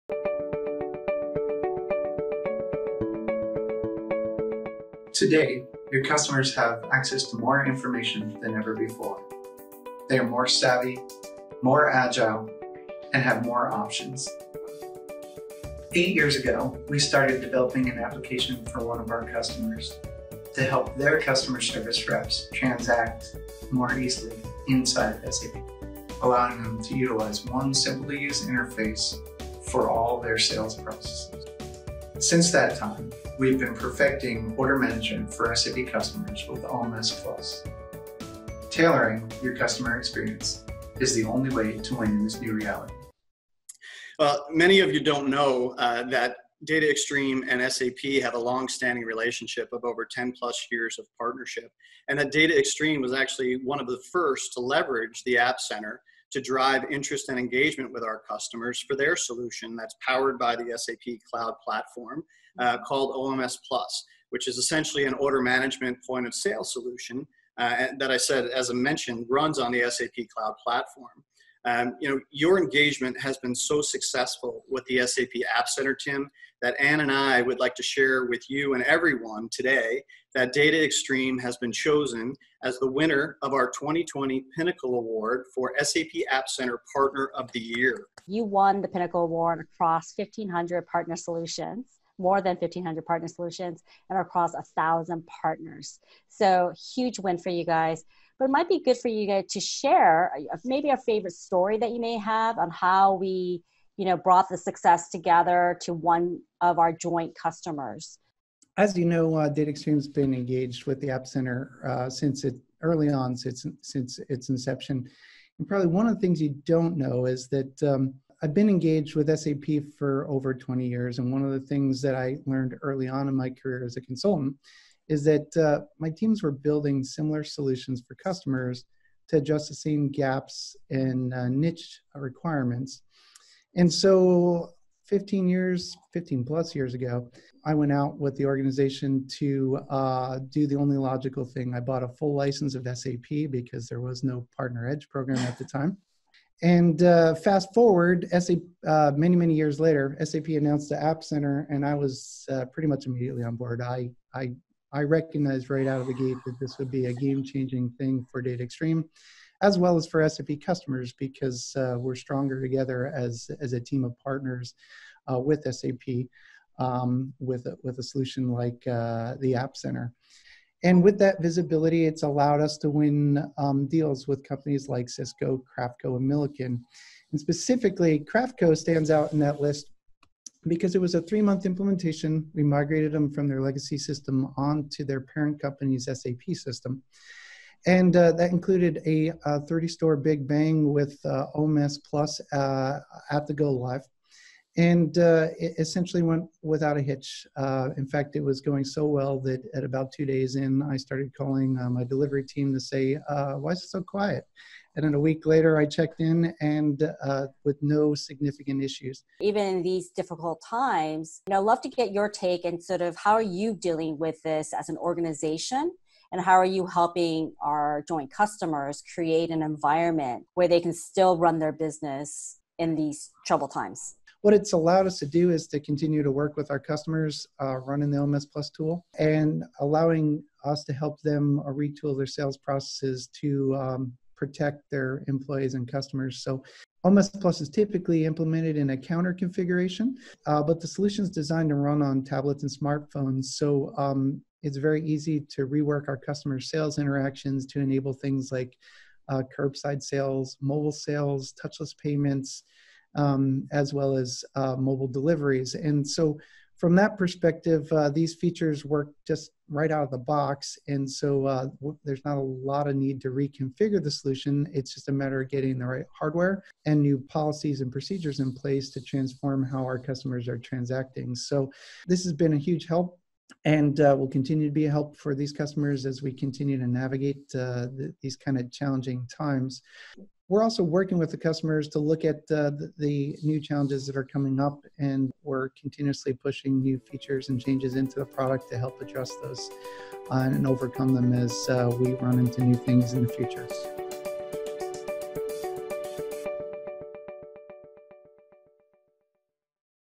Intro to DataXstream Sapphire 2020
DataXstream Sapphire 2020 - Originally created for Sapphire Converge 2020, the re-imagined online event to replace SapphireNow Orlando. This is an introduction to DataXstream, an SAP Partner and long time participant in SapphireNow and ASUG events. In this intro we also highlight DataXstream's OMS+ solution.

OMS+ is an SAP certified solution found in the Sap App Center.
It is an order management / point of sale solution created to make selling efficient and to enable better customer experiences. OMS+ leverages SAP allowing users to quickly and easily create orders, shipments, and deliveries with real time data. Designed specifically for SAP, OMS+ does not require integration, can be implemented in weeks, and requires less than a week for user training.